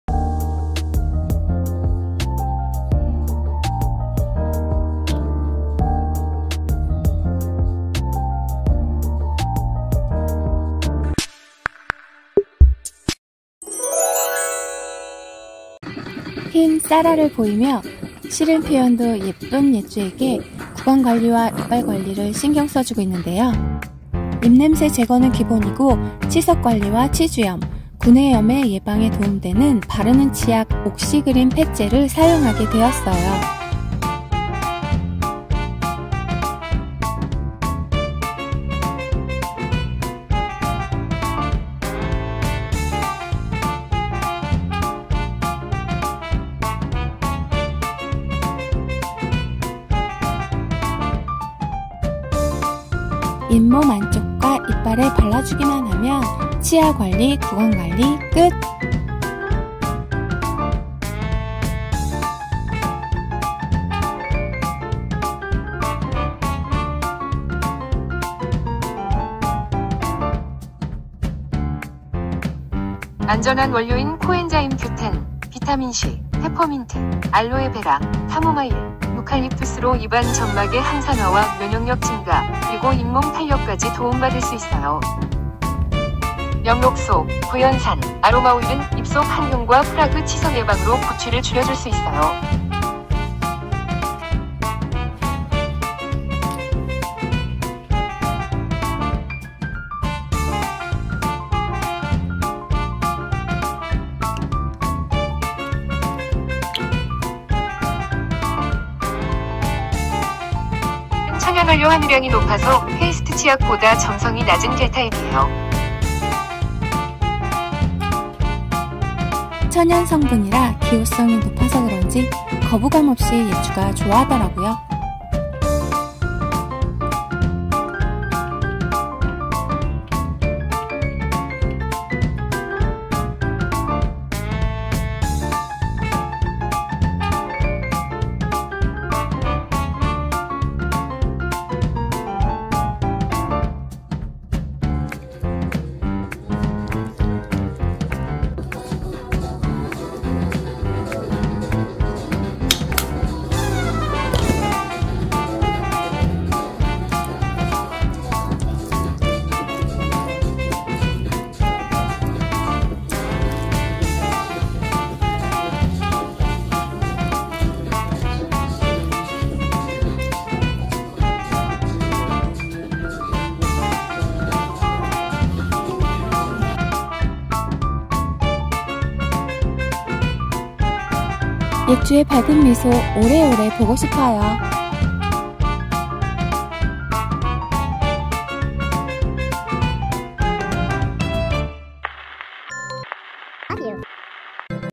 Yejjyu's tried(67)🪥 강아지양치 힘들었다면! 바르는 잇몸염증치약 입냄새까지 싹~ 강아지치약 추천
전쟁같은 양치 시간 이제 그만!
강아지가 좋아하는 치약이면서 잇몸염증 예방까지

고양이잇몸치약, 강아지입냄새, 강아지치아영양제
고양이치약추천 오세인트 고양이치약